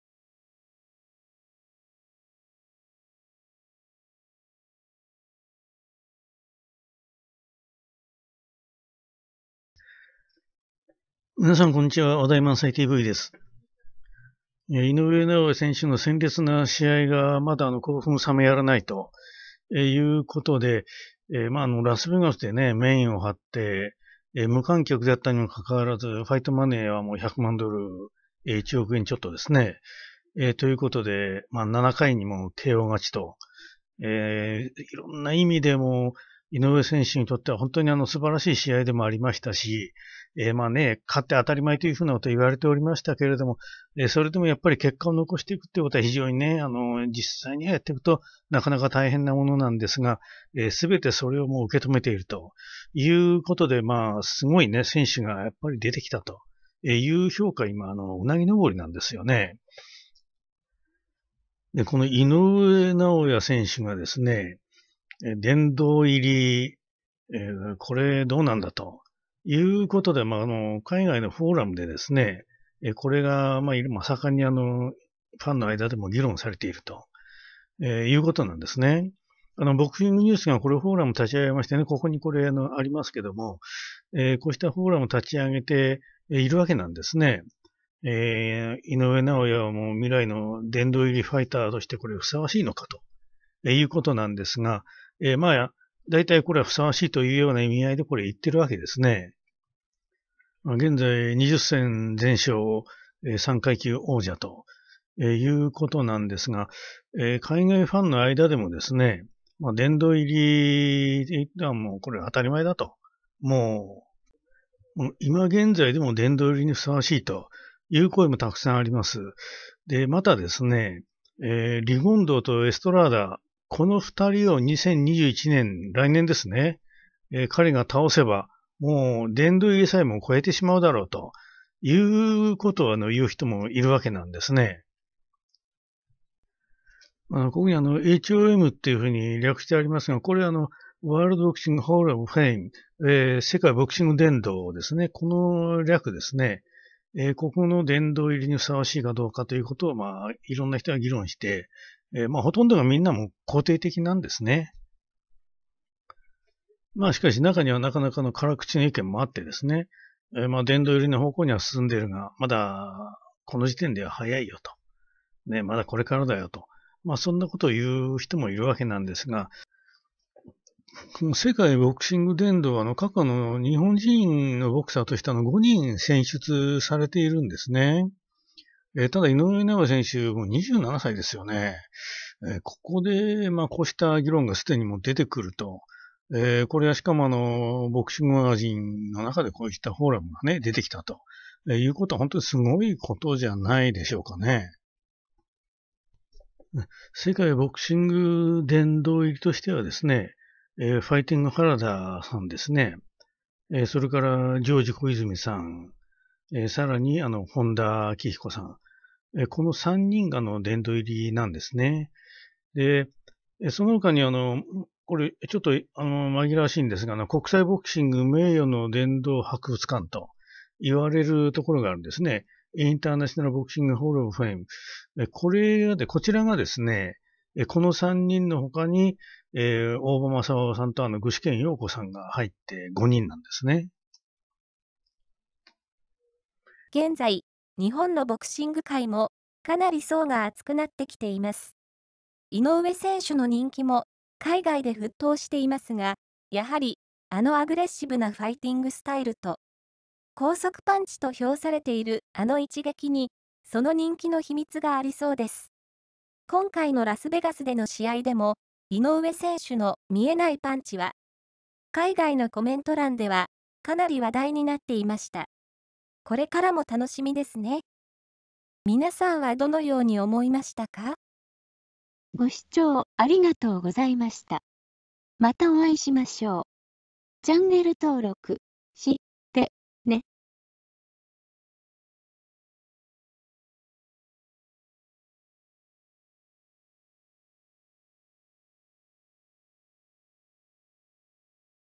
井上尚弥 早くも殿堂入りか？ もし来年…すれば殿堂入りさえ超えるぞ！との声が、海外ファンの間で湧き上がる モンスター・イノウエ
井上尚弥　早くも殿堂入りか？
ボクシングマガジンフォーラム
もし来年…すれば殿堂入りさえ超えるぞ
との声が海外ファンの間で湧き上がる
Monster Inoue

登録するとちょっぴり知的に楽しめるチャンネルです
当チャンネルは時事雑学などを中心としています。
■画像元/参照記事
BOXING　NEWS　24